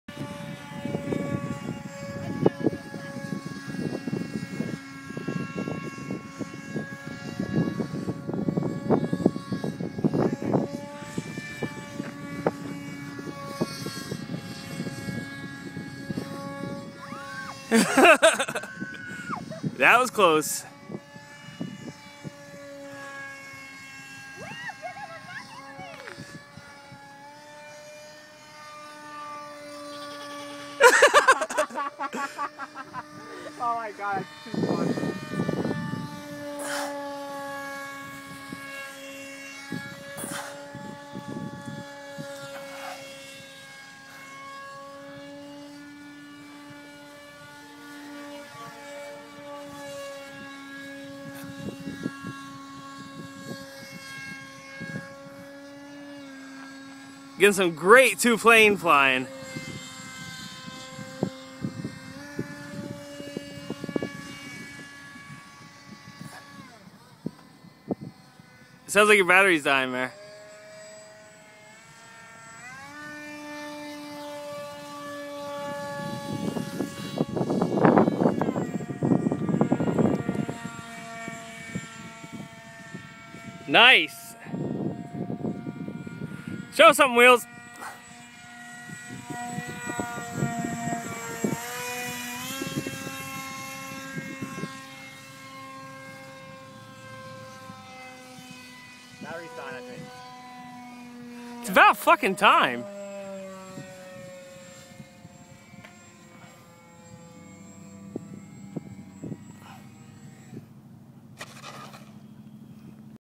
Two planes in the air!
Ryan and Mary flying around. This is video from after the battery died in my F22.